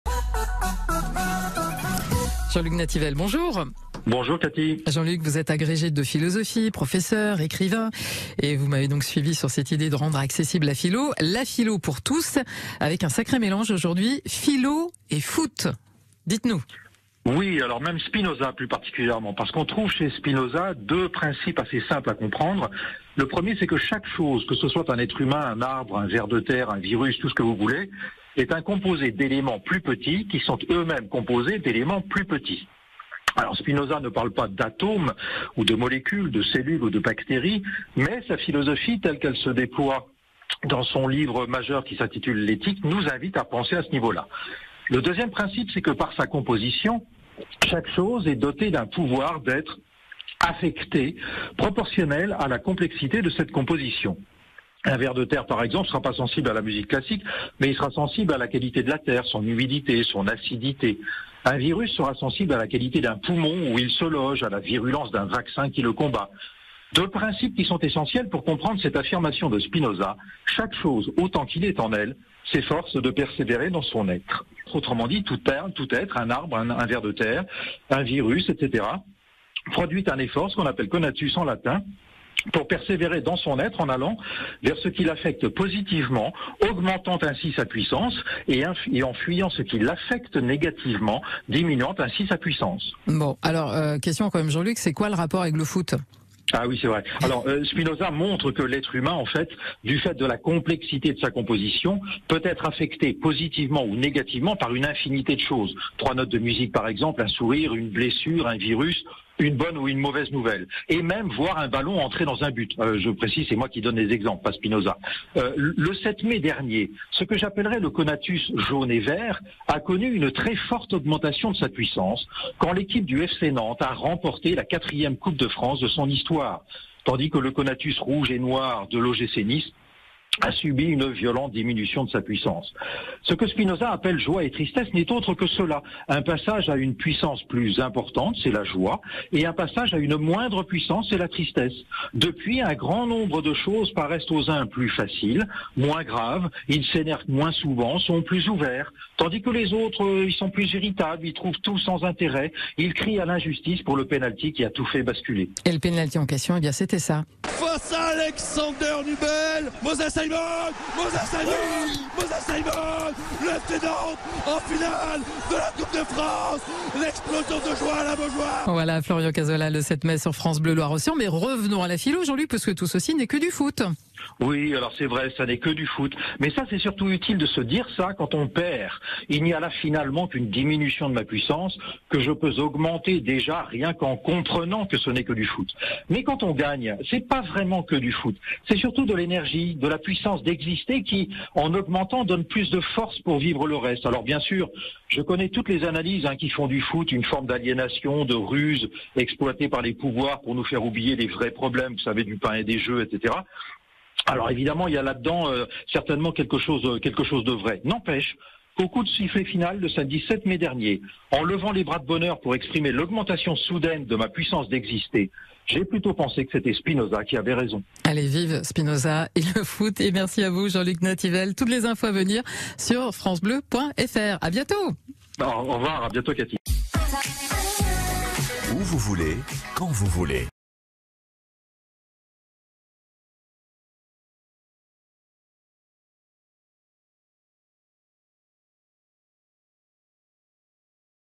SPINOZA et le football, Jean-Luc Nativelle
Présentation : Sans le savoir forcément, lorsque nous levons les bras d'enthousiasme ou que nous baissons la tête de déception, nous appliquons concrètement l'un des principes centraux de la philosophie de Spinoza : "chaque chose, autant qu'il est en elle, s'efforce de persévérer dans son être". Le 7 mai dernier, le supporter nantais que je suis - on a les défauts qu'on peut... - a vu sa puissance d'exister augmenter sensiblement quand le FC Nantes a remporté la 4e Coupe de France de son histoire. On dira sans doute que ce n'est que du foot. Mais Spinoza nous aide à comprendre que c'est peut-être un peu plus que ça, surtout quand on gagne...

Jean-Luc Nativelle est professeur agrégé de philosophie, enseignant en classes préparatoires aux grandes écoles à Angers, il intervient également à l'Université Permanente de Nantes.

Jean-Luc Nativelle est notamment l'auteur de Une lecture de Spinoza, 5 clés pour entrer dans l'Éthique aux Editions M-Editer